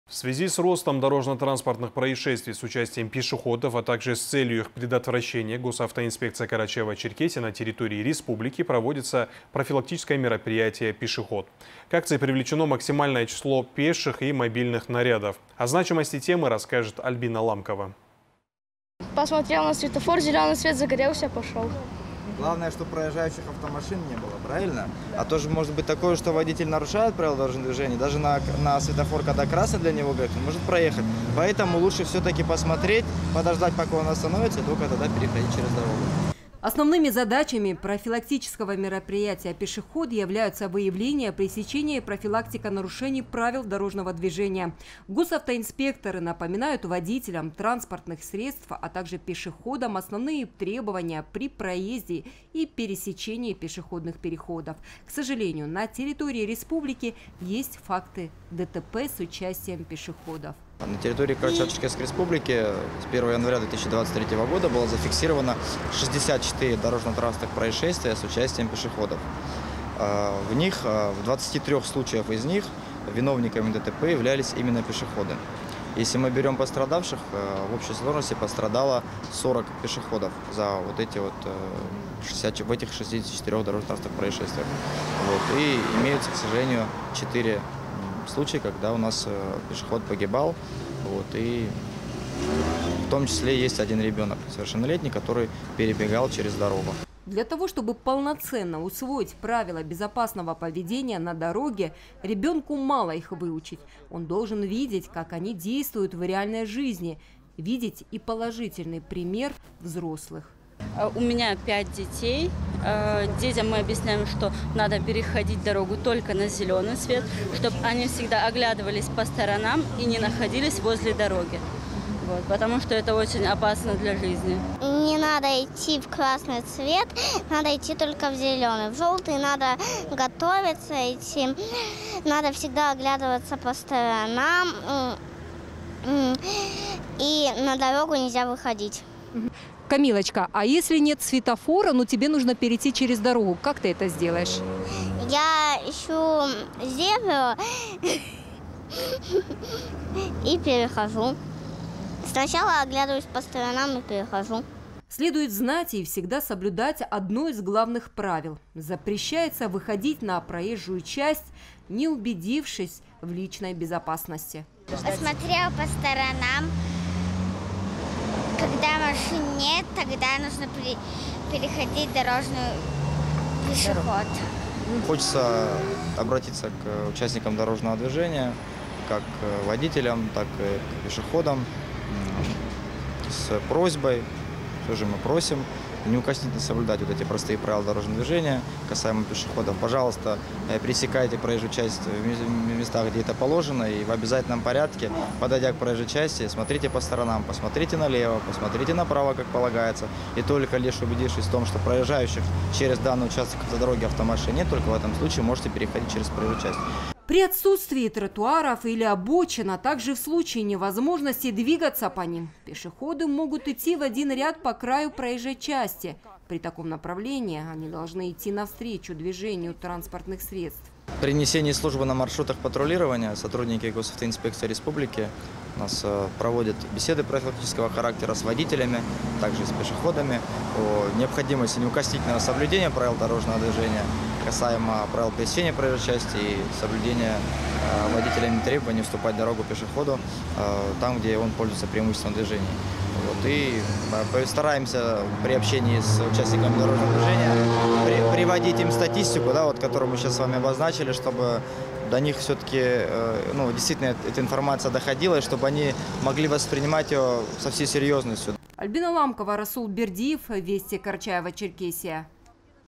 ГИБДД Карачаево-Черкесии проводит акцию "Пешеход"
В связи с ростом дорожно-транспортных происшествий с участием пешеходов, а также с целью их предотвращения, Госавтоинспекцией Карачаево-Черкесии на территории Республики проводится профилактическое мероприятие "Пешеход". К акции привлечено максимальное число пеших и мобильных нарядов